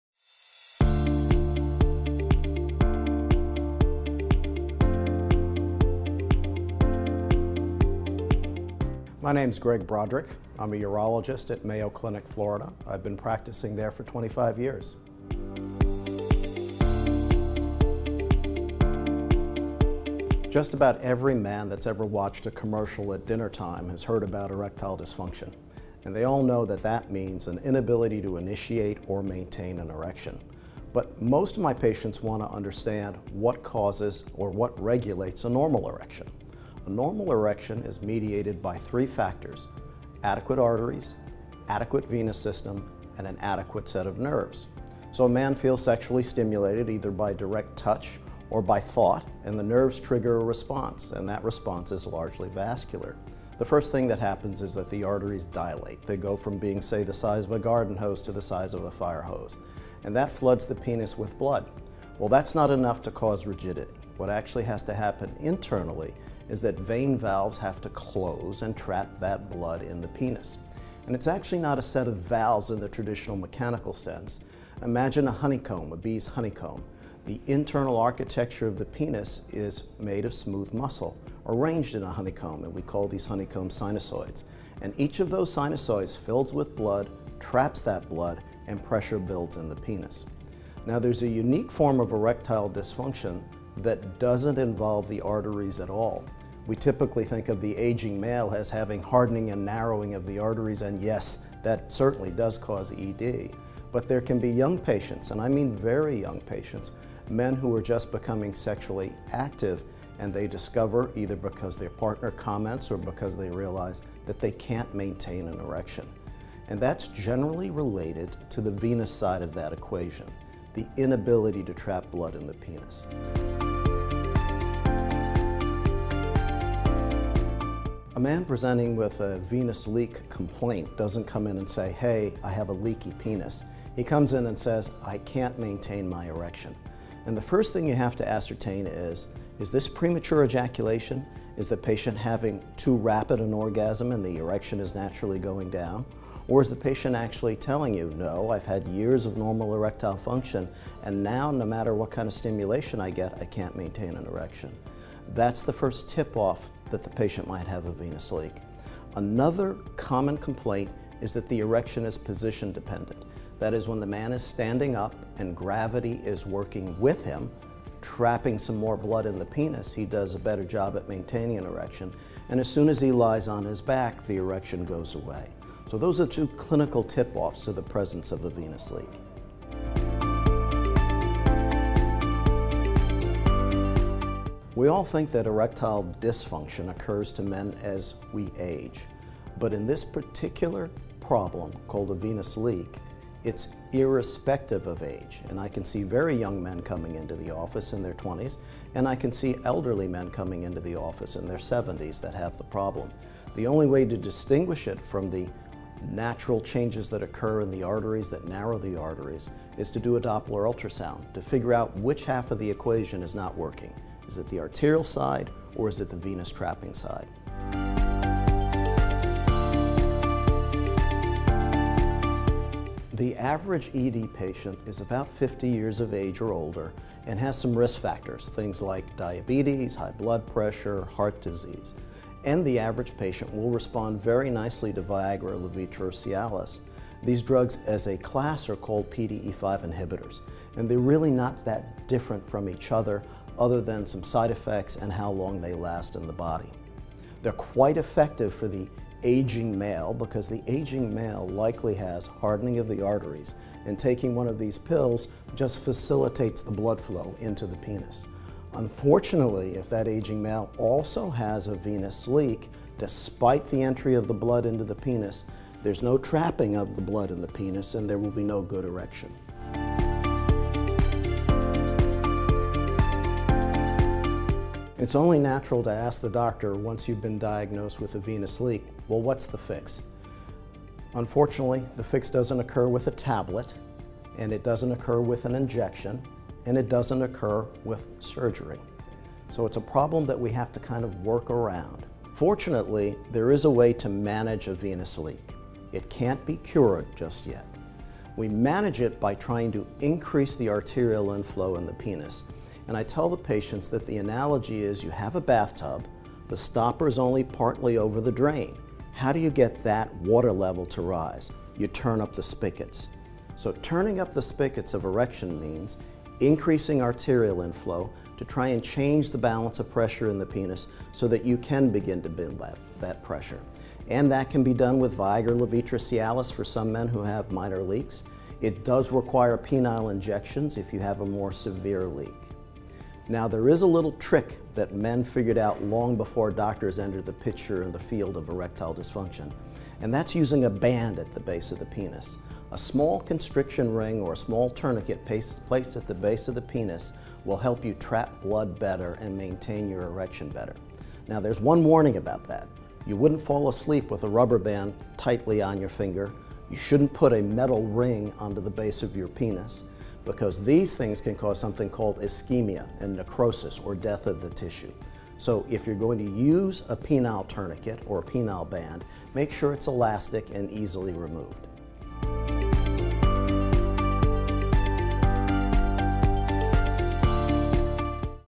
What Is Venous Leakage?
What Is Venous Leakage? Ask a sexpert! Dr. Greg Broderick answers this question in the latest installment of this video series.
👥 Access exclusive member benefits from the SMSNA.

The objective of the SMSNA is to promote, encourage, and support the highest standards of practice, research, education, and ethics in the study of human sexual function and dysfunction.